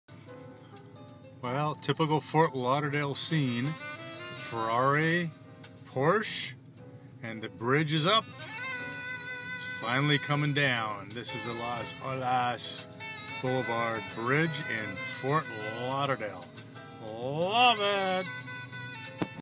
Youtube video of Las Olas bridge open for vessel to pass. Love Ft Lauderdale
YOutube video of the Las Olas bridge open for vessel to pass. I Love Ft lauderdale, FL!!

Randall Burg